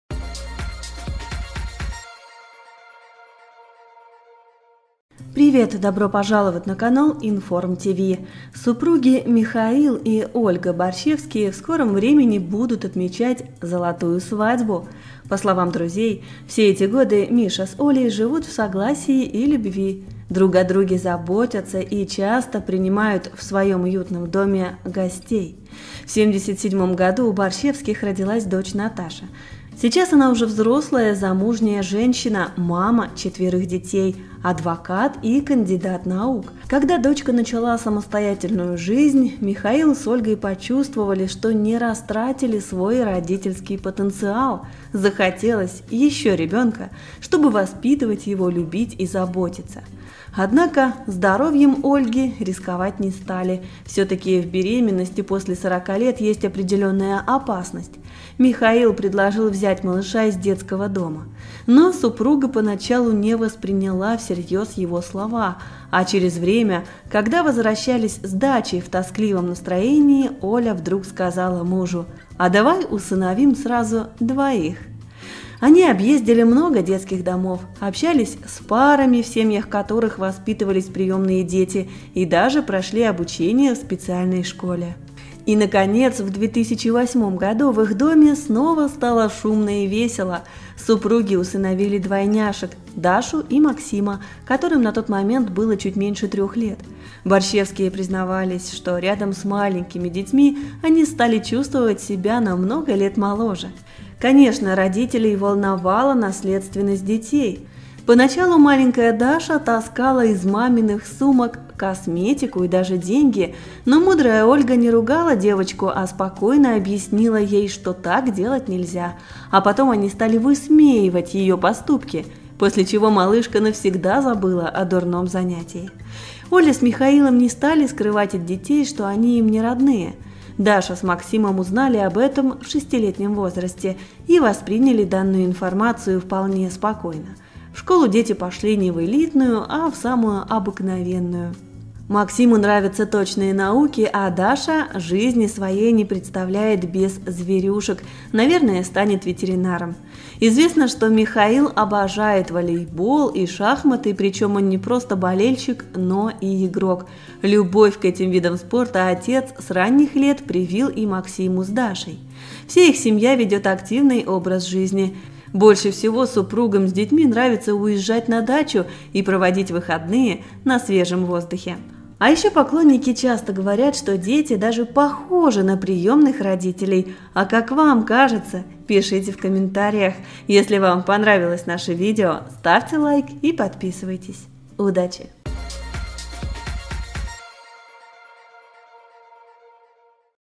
Как сейчас выглядят ПРИЁМНЫЕ ДВОЙНЯШКИ Михаила Барщевского, которых он усыновил в 53 года
Супруги Михаил и Ольга Барщевские в скором времени будут отмечать золотую свадьбу. По словам друзей, все эти годы Миша с Олей живут в согласии и любви, друг о друге заботятся, и часто принимают в своем уютном доме гостей.
В 1977 году у Барщевских родилась дочь Наташа. Сейчас она уже взрослая замужняя женщина, мама четверых детей, адвокат и кандидат юридических наук. Когда дочка начала самостоятельную жизнь, Михаил с Ольгой почувствовали, что не растратили свой родительский потенциал. Захотелось еще ребенка, чтобы воспитывать его, любить, заботиться.